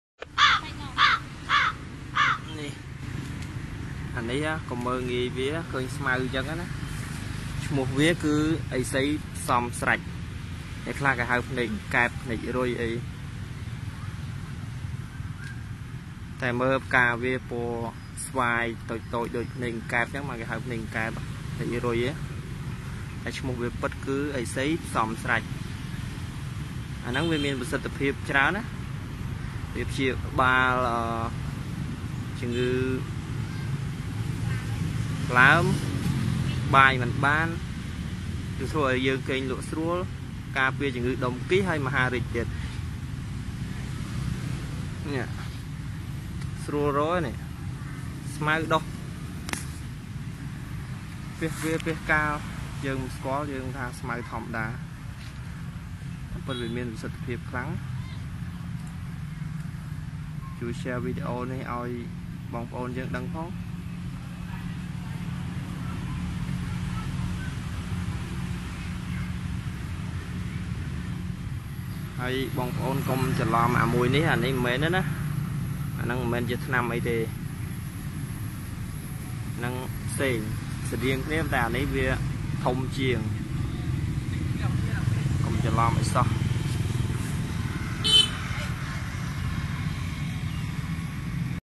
Anti Cancer Weed ឥសីផ្សំស្រេច ឬ ហៅថា ភ្នែករុយ
This kind of weed is very easy to find in Cambodia, it spread any where in the country but not many percentage of people know how useful of it. Today, I would like to show you the advantage of it. Please, watch this video first and if you have any question, do not hesitate to drop a COMMENT bellow or send me a message then I will reply to you as soon as possible. I hopw my video can help people from the Cancer but only just cancer it can anti anykind of Cyst, make you feel to eat and sleep better too.
I love you all my dear friends!

ឥសីផ្សំស្រេច ជាស្ដេចថ្នាំមួយប្រភេទដែលយើងតែងមើលរំលង តែវាពិតជាមានប្រសិទ្ធិភាពខ្ពស់ណាស់សំរាប់ព្យាបាលជំងឺ មហារីក ដុំគីស ថ្លើម បាយមិនបាន គេងមិនលក់ជាដើម។

សូមមេត្តាជួចែករំលែកវិដេអូនេះដើម្បីជាប្រយោជន៍ដល់ប្រជាជនយើងផង
សូមអរគុណ